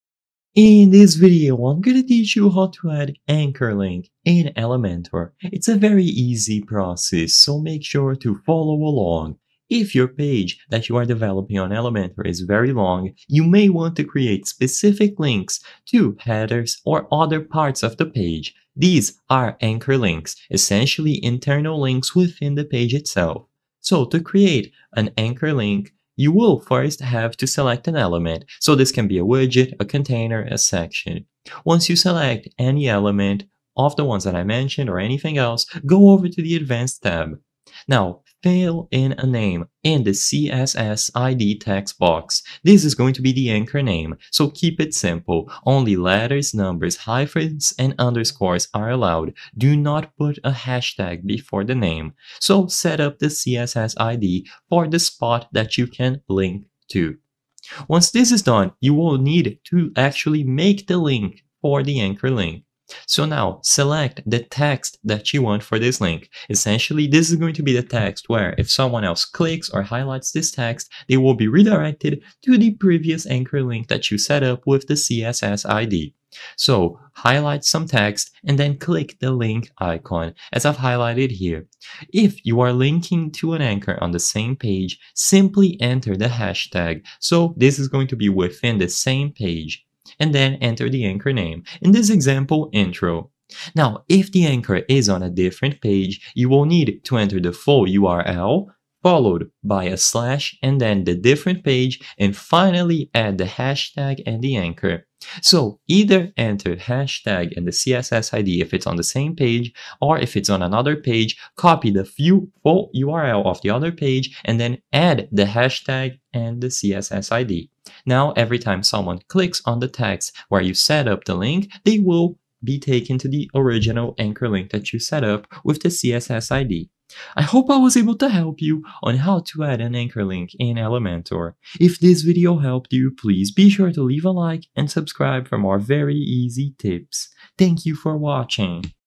How To Add Anchor Link in Elementor (2025) - Step by Step
In this video I will clear your doubts about how to add anchor link in elementor, and whether or not it is possible to do this.